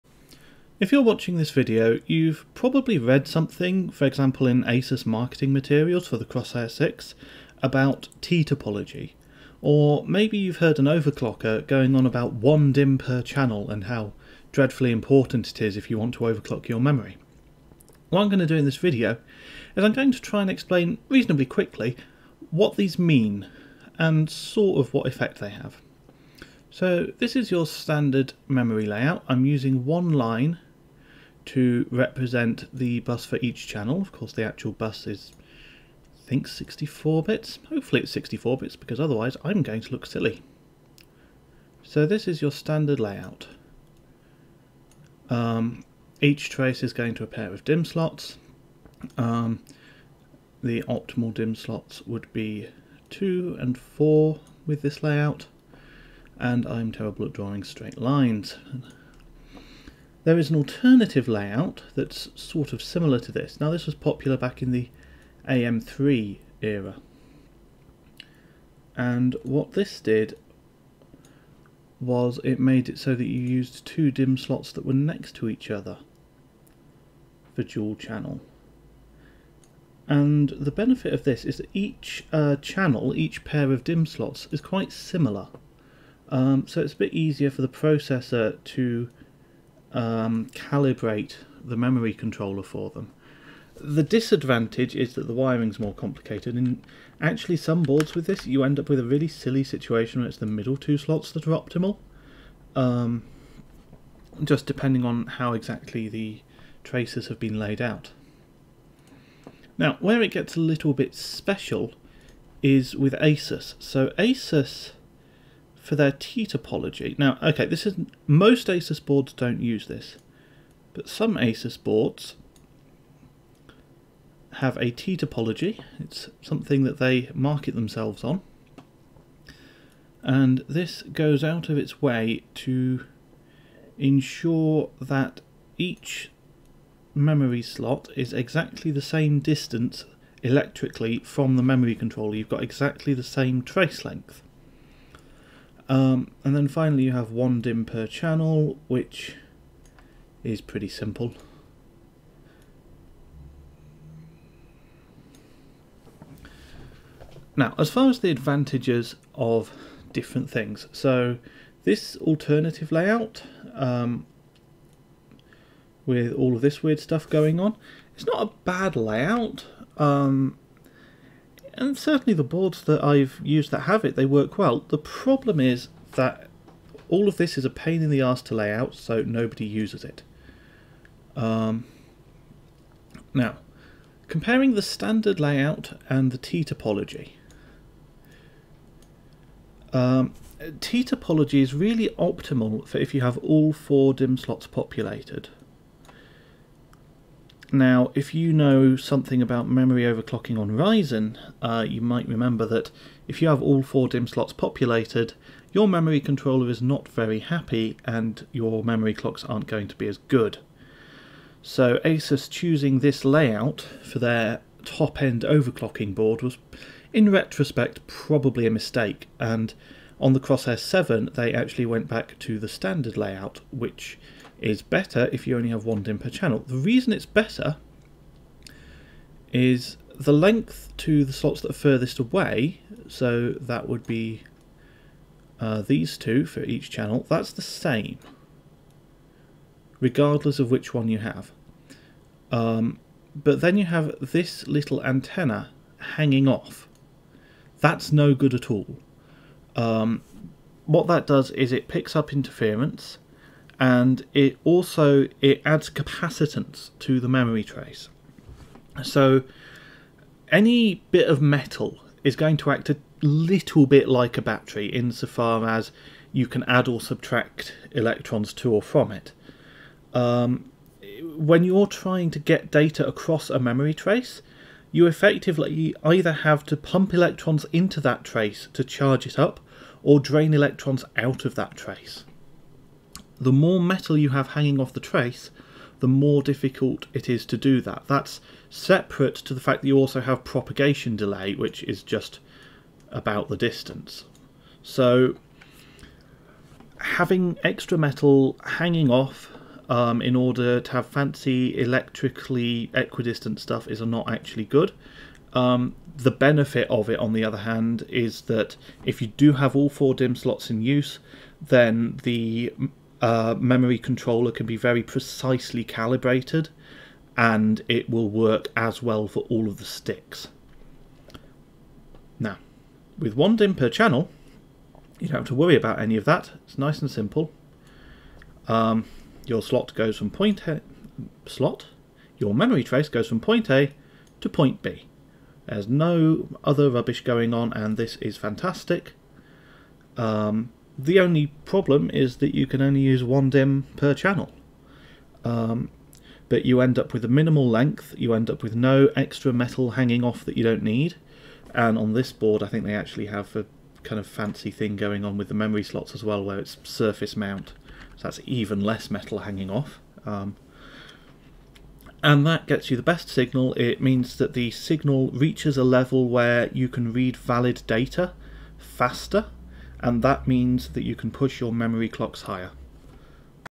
What is T-Topology and benefits of 1 DIMM per channel in about 6 minutes
complain about how loud the mouse clicks are in the comments

I try to quickly go over the benefits and drawbacks of different memory trace layouts, explaining why 1 DIMM per channel is best.